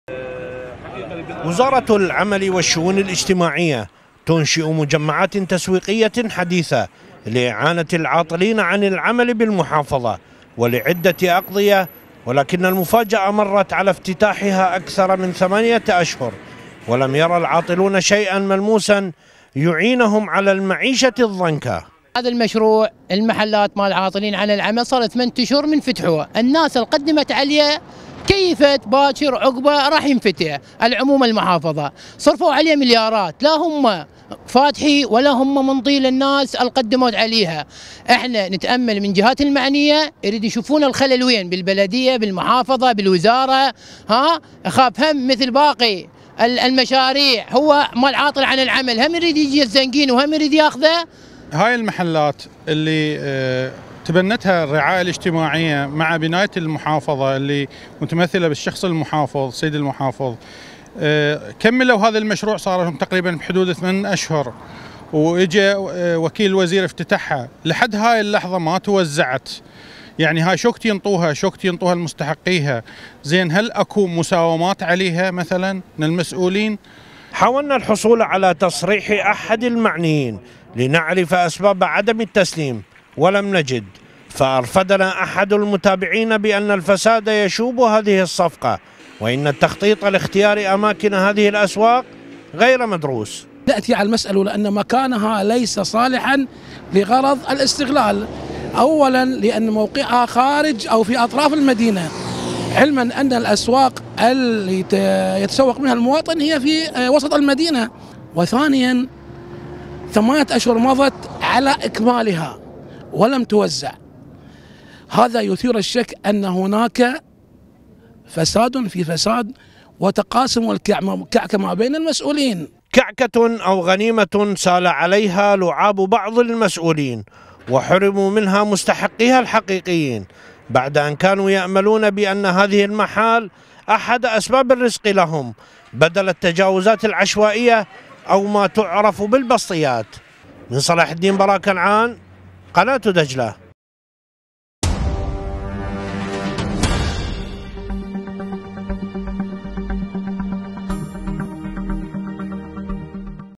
شكاوى في صلاح الدين من عدم تسليم المجمعات التسويقية : "المـساومات حـاضرة بـقـوة"
10891📺 افقي
10892📺 افقي
10893📺 افقي